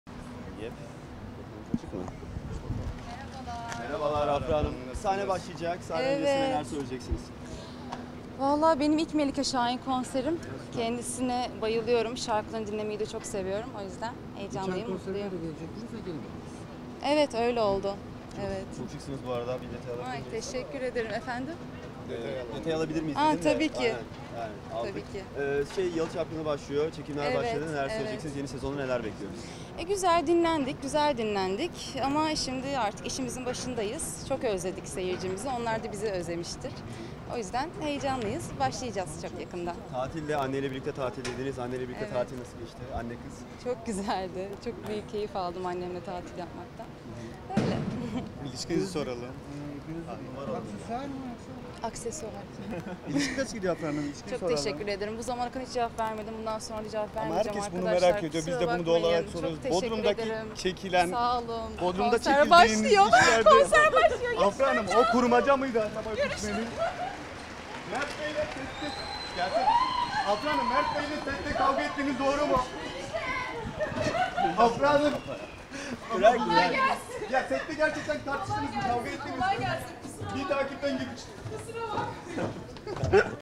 Afra Saraçoğlu Koşa Koşa Kaçtı
Afra Saraçoğlu, Mert Ramazan Demir ile ilgili soruyu duyunca kaçtı!

TÜRKSAT 4A
Frekans: 12729
Sembol: 30000
Polarizasyon: V (Dikey)